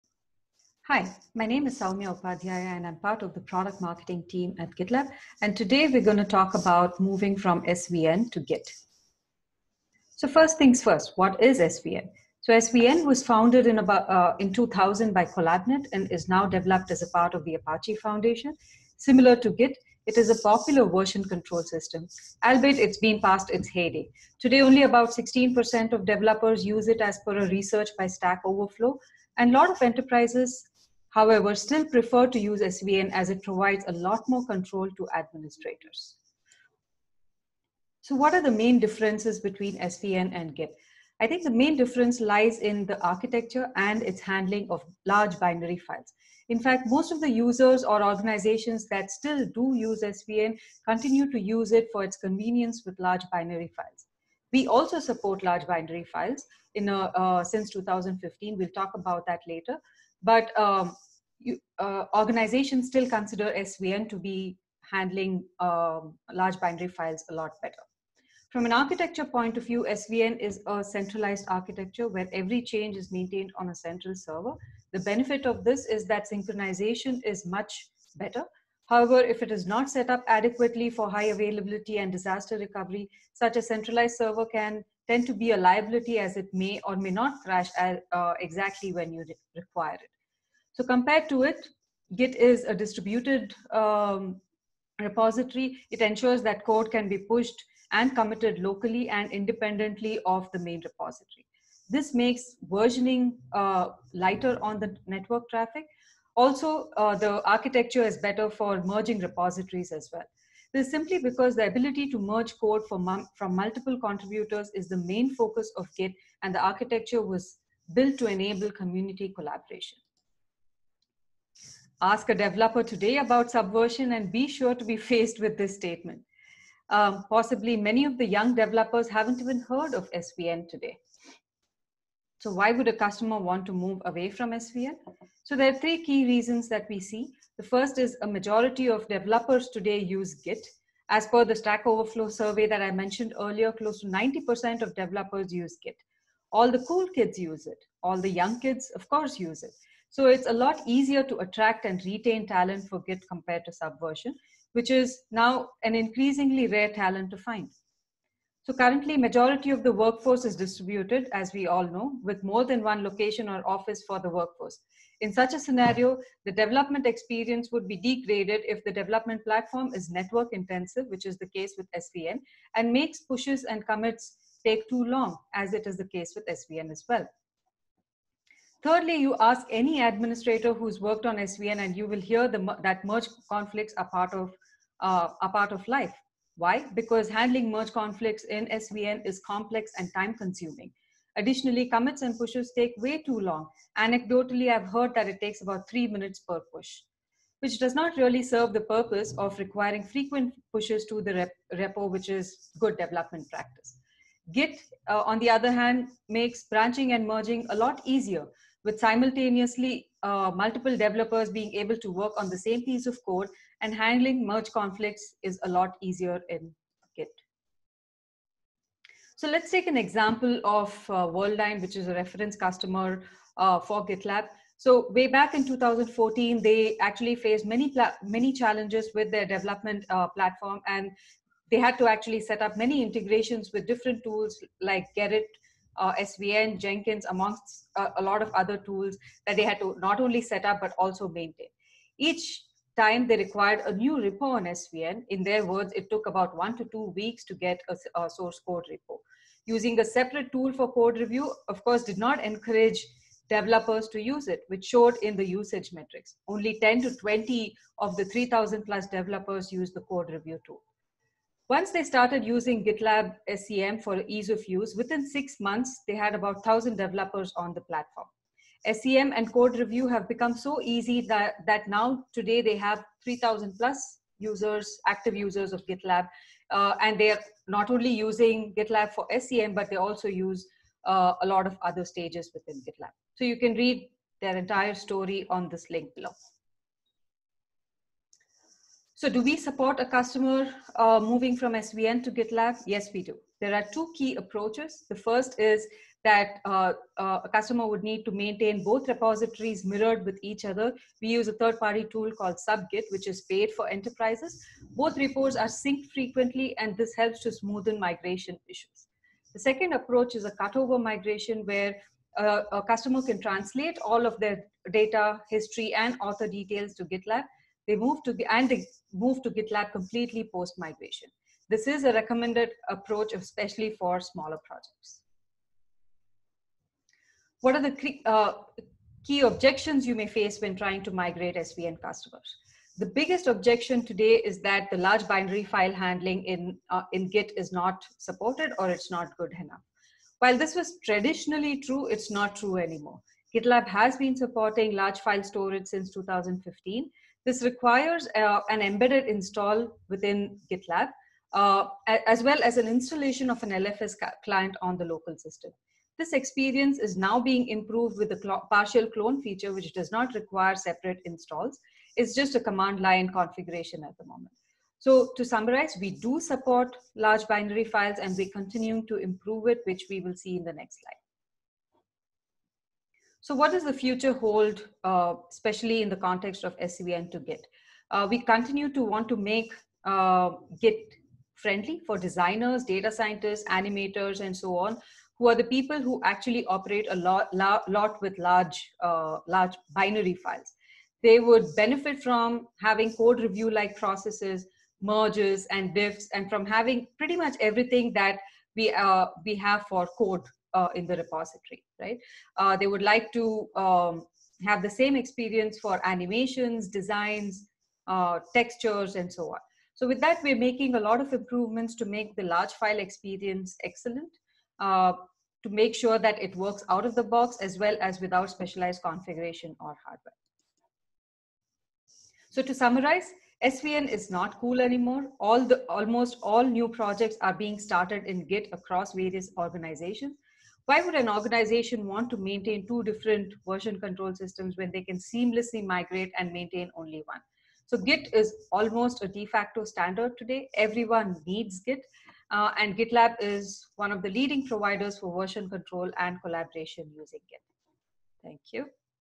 Sales Level Up: Moving from SVN to Git
Quick video to educate SALs on SVN and moving from SVN to Git.